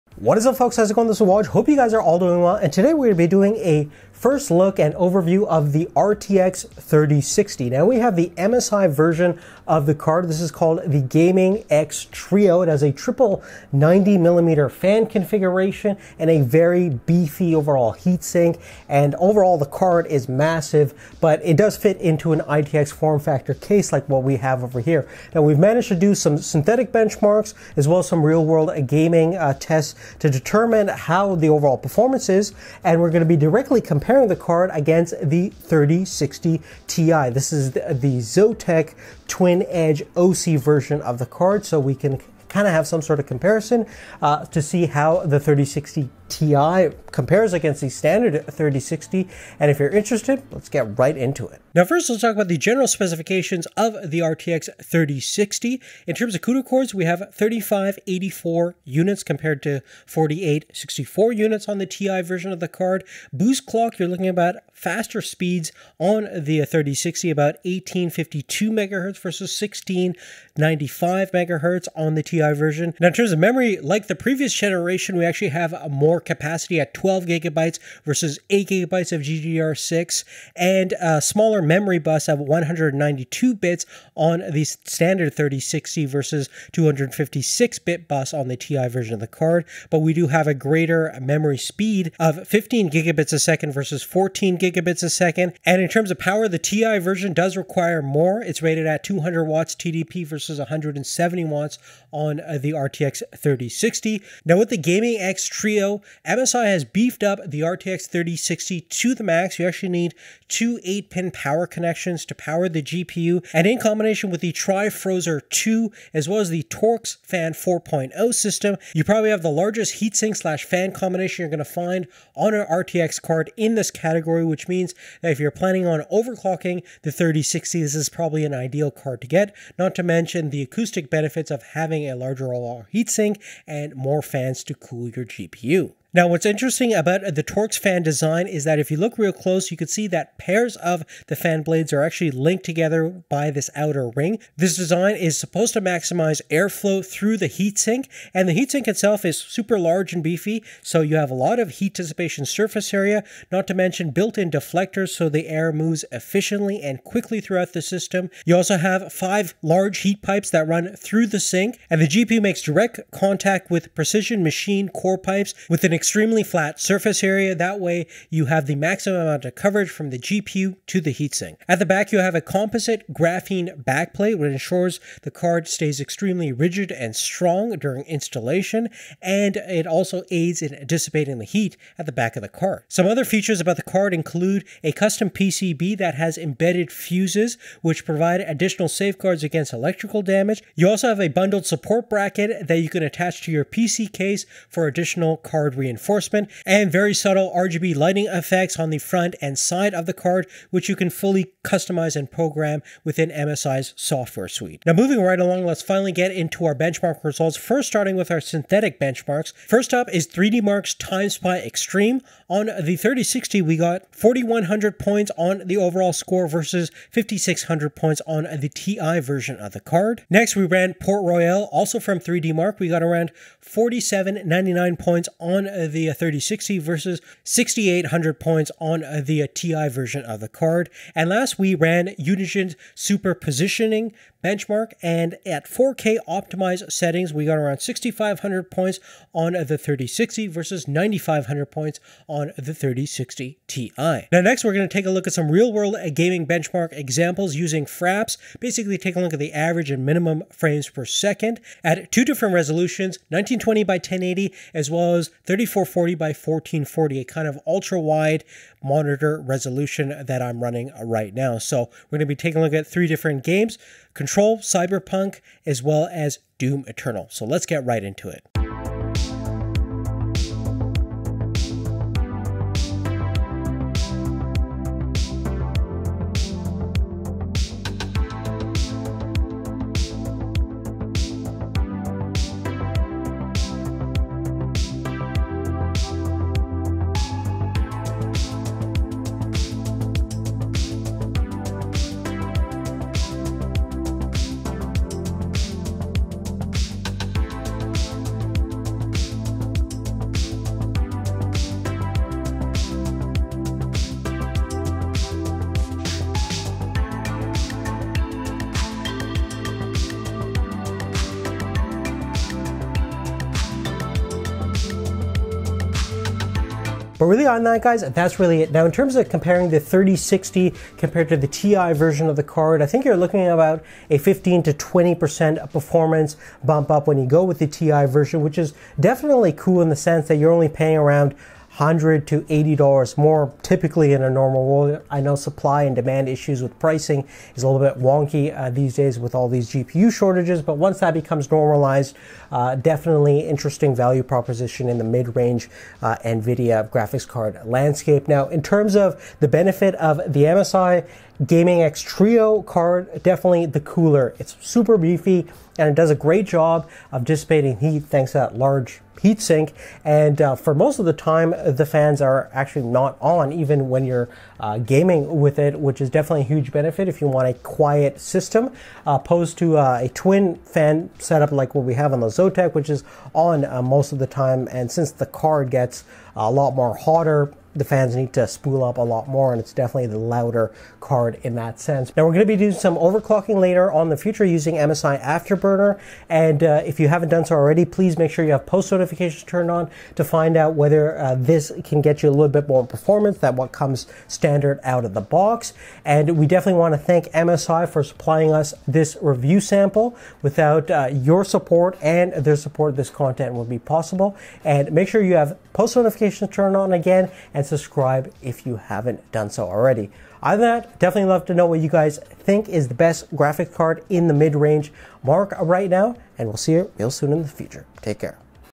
MSI GeForce RTX 3060 GAMING X TRIO 12G- Overview & Initial Benchmarks
Here is our overview and initial benchmarks of the MSI GeForce RTX 3060 GAMING X TRIO 12G.

PC Specs:
CPU- AMD Ryzen 7 5800x
GPU: ZOTAC GAMING GeForce RTX 3060 Ti Twin OC
Mobo- B550I AORUS PRO AX
RAM: Corsair LPX
SSD: WD Black SN750
Cooler: DeepCool Castle 120R AIO Liquid
SILVERSTON 500W SFX PSU